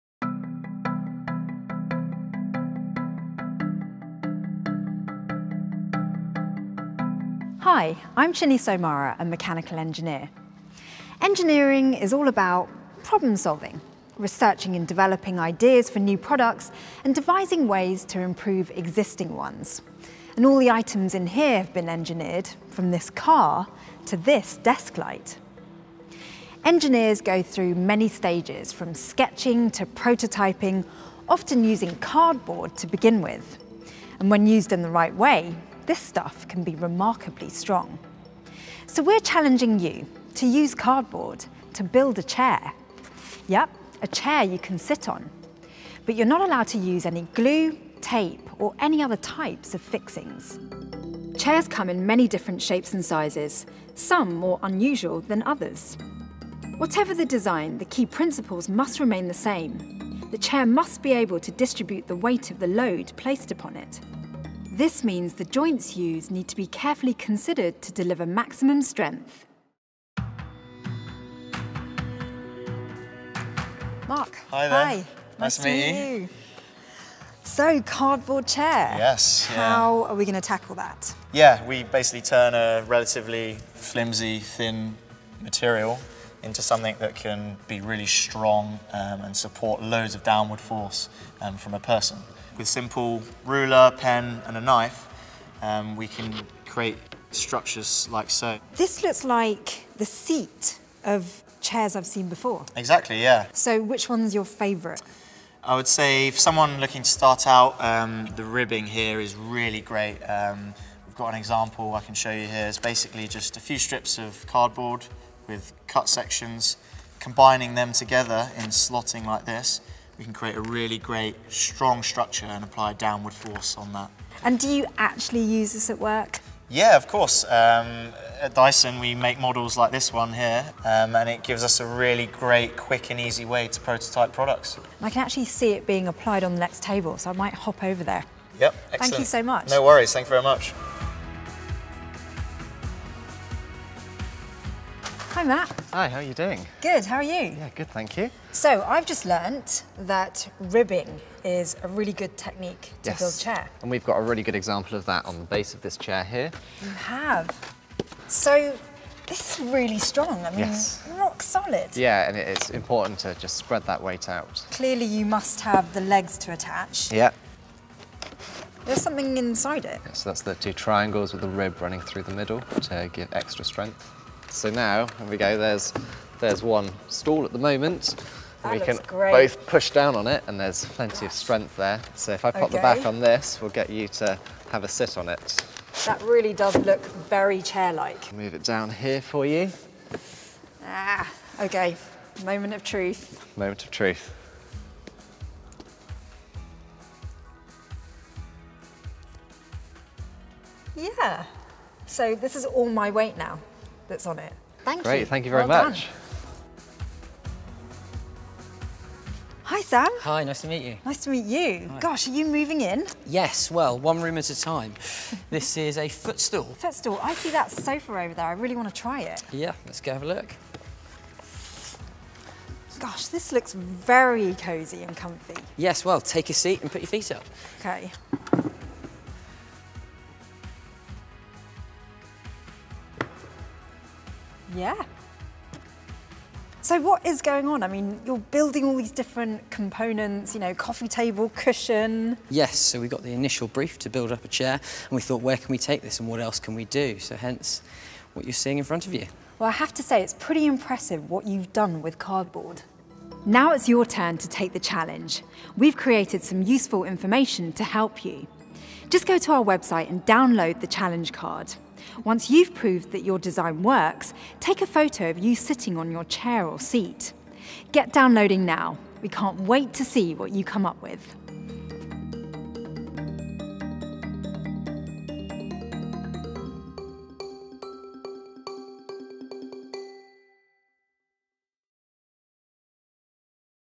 Educational Activities for Kids: Cardboard Chair
Can you make a chair from cardboard? Fun, hands-on educational activities for kids using household materials.

Watch the video to see find out more and see Dyson engineers take on the challenge.